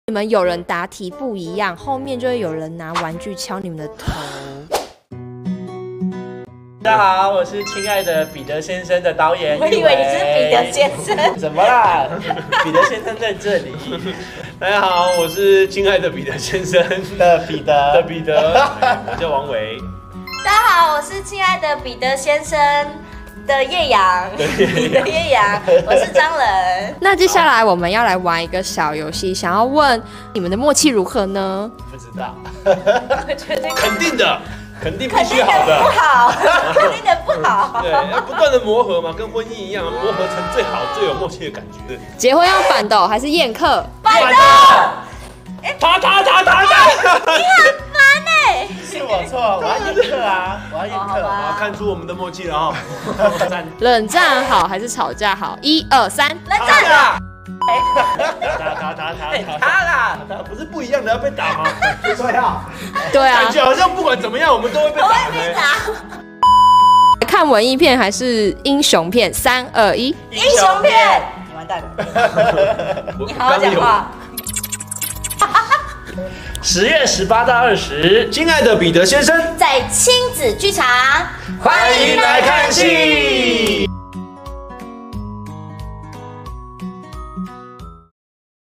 💗親愛的彼得先生💗 獨家爆笑拍攝👏 默契大考驗part1❗️
💗親愛的彼得先生💗 獨家爆笑拍攝👏
🎬 齊聚導演和兩位演員 默契大考驗part1❗️
一言不合就扒頭？
三人究竟是彼此的 神隊友 👑
還是 不如一句say估掰 的雷隊友呢
王為：默契肯定好的！不斷磨合～跟 婚姻💍 一樣 會磨合成最好最有默契的感覺🫶
笑的不夠過癮？
走進劇場 解鎖更多 令你哭笑不得的 婚姻百味啦🤭
臺北市政大樓親子劇場
10/18（五） 19:30
10/19（六） 14:30、19:30
10/20（日） 14:30